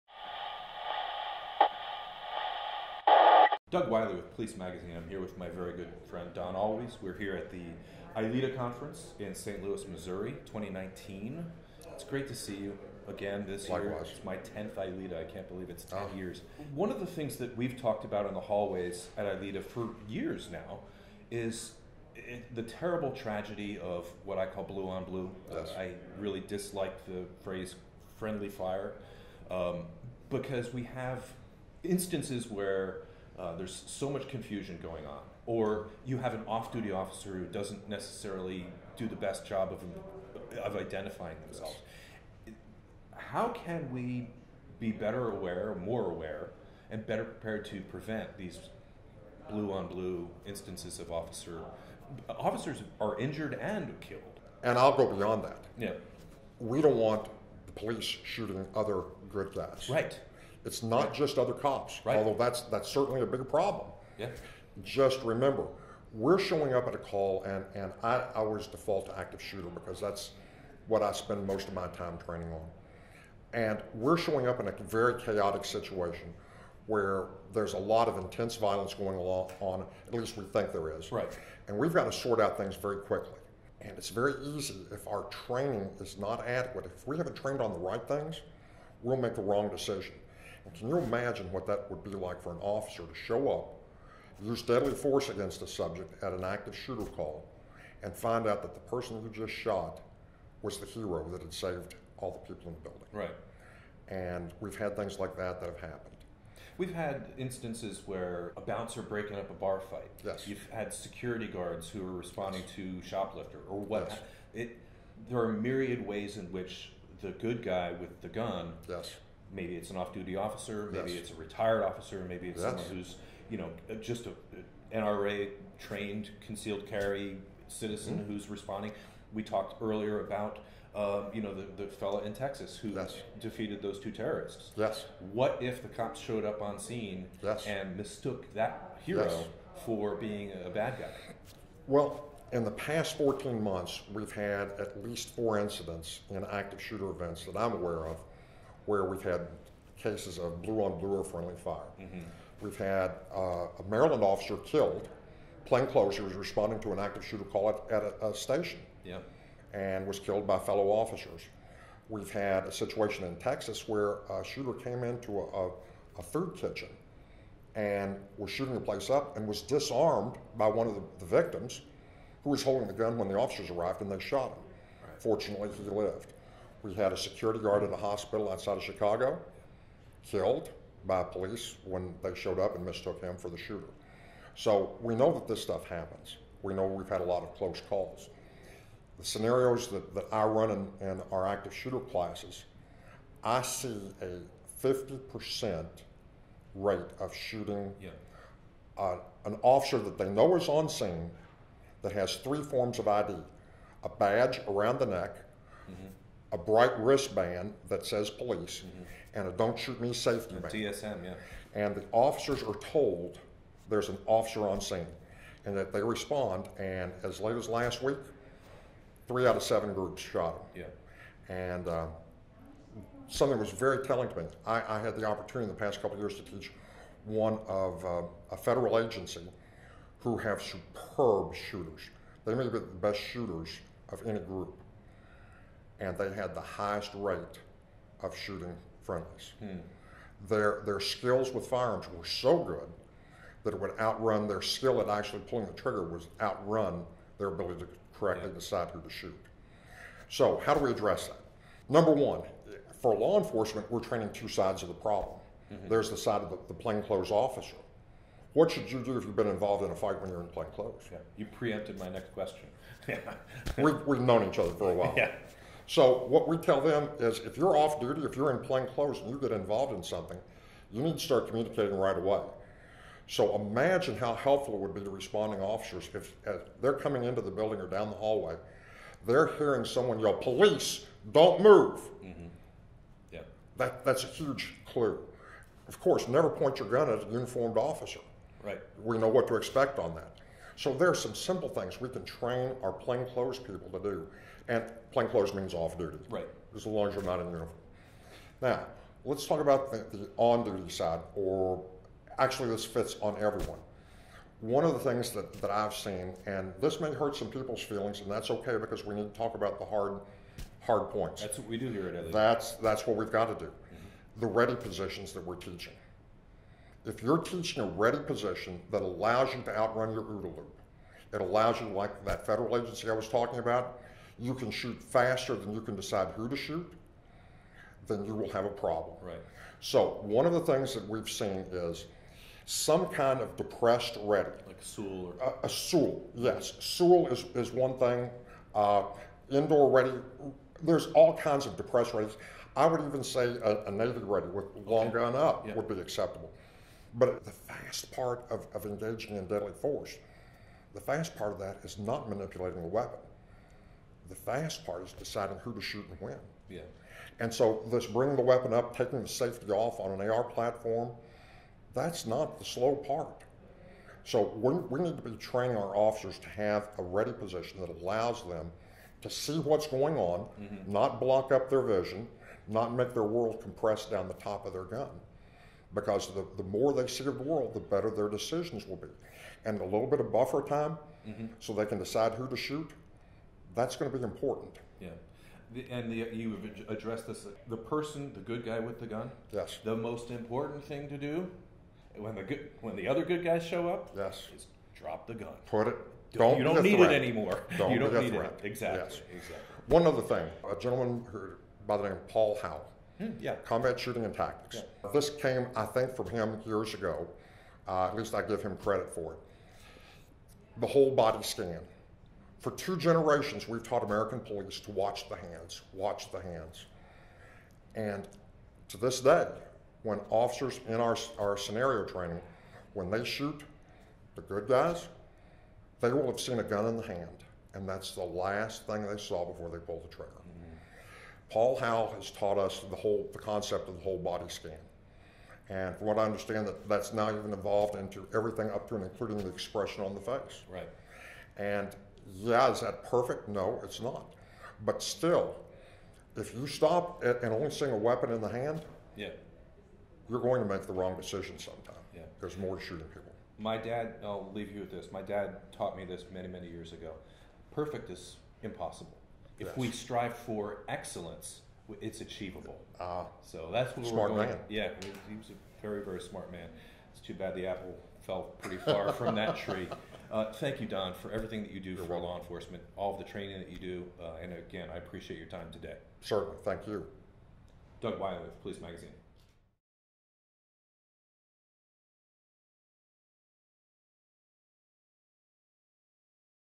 Mitigating the Tragedy of Blue-on-Blue Deaths
Don Alwes—an adjunct instructor for Kentucky's Department of Criminal Justice Training and lead instructor for NTOA's Police Response to Active Shooter and School and Workplace Violence courses—discusses with PoliceMag.com contributing web editor Doug Wyllie the problem of blue-on-blue accidental shootings. Alwes stresses the need for plainclothes or off-duty officers to identify themselves to arriving uniformed officers, and to immediately comply with commands.